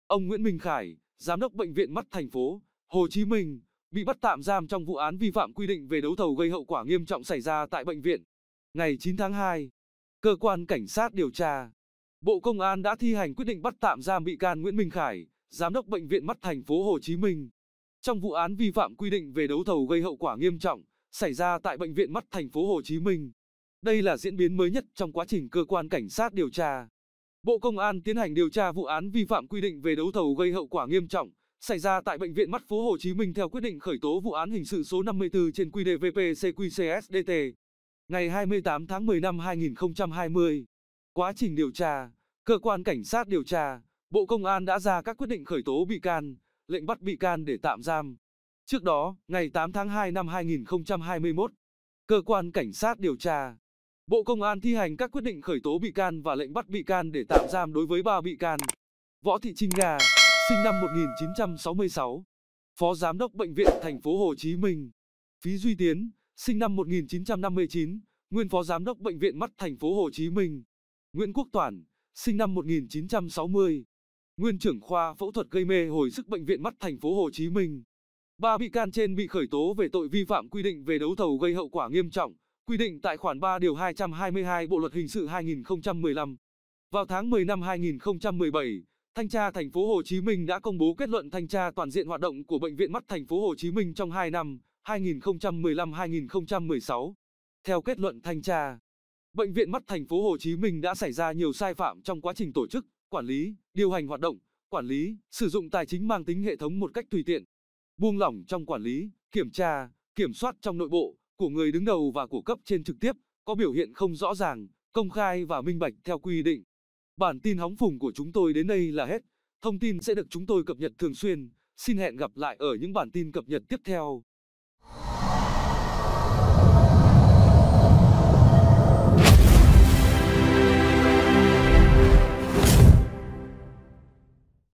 Bắt tạm giam Giám Đốc bệnh Viện Mắt TP HCM [Hóng Phùng]
Ông Nguyễn Minh Khải (giám đốc Bệnh viện Mắt TP.HCM) bị bắt tạm giam trong vụ án 'Vi phạm quy định về đấu thầu gây hậu quả nghiêm trọng' xảy ra tại bệnh viện.
Theo: Tuổi Trẻ